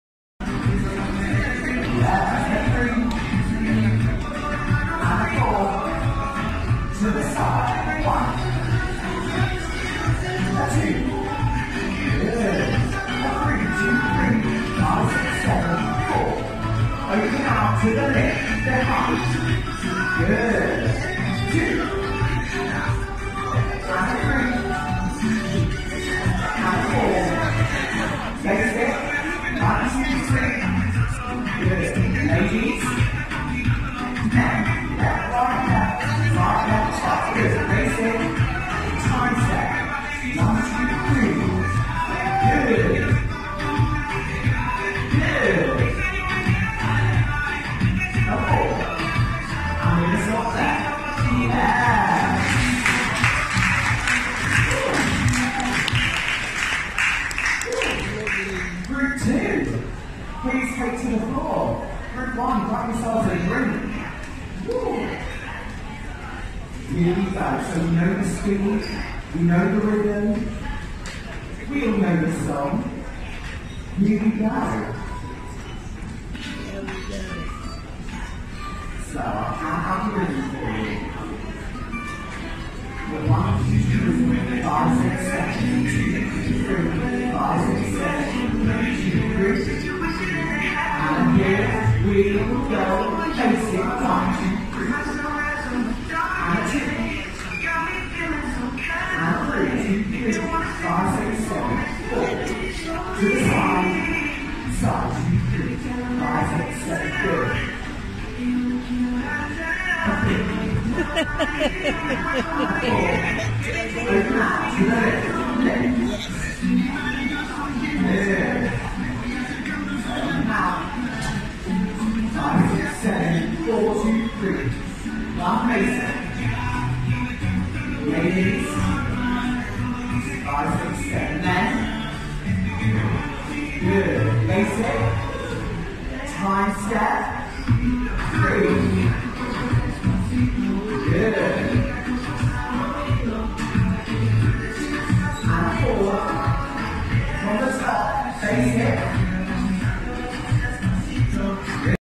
Learn to dance onboard the P&O Ventura cruise ship
What better place to learn to dance than on a cruise ship. Fantastic. So much to do on the Ventura she is truly an amazing vessel. It's hard to believe that your floating.its super posh and all the staff are so friendly. If you've never cruises before then do t wait a minute longer. Ask me anything if I can help I will. Cheers Louise. X